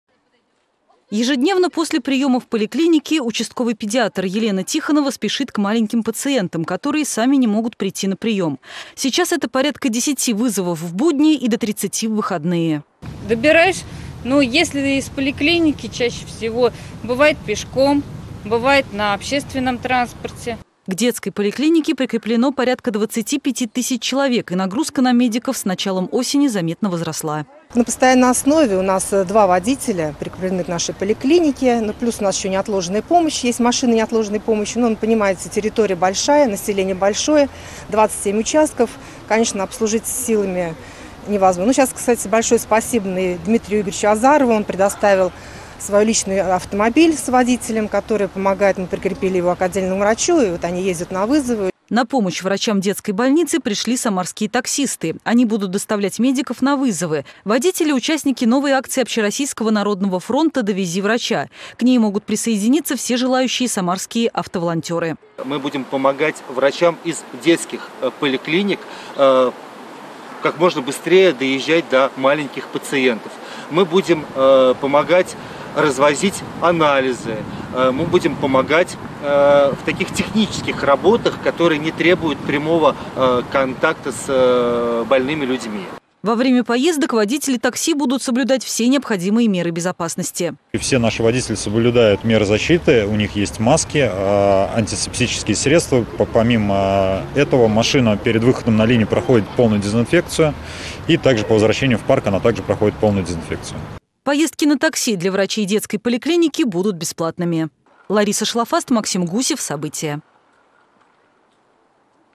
На помощь врачам придут таксисты и волонтеры
Помогать медикам будут автоволонтеры. В Самаре Общероссийский народный фронт запустил новую акцию — добровольцы могут доставлять анализы в лаборатории из клиник медицинского университета. На помощь врачам приходят и транспортные предприятия. Подробнее в сюжете Ларисы Шалафаст.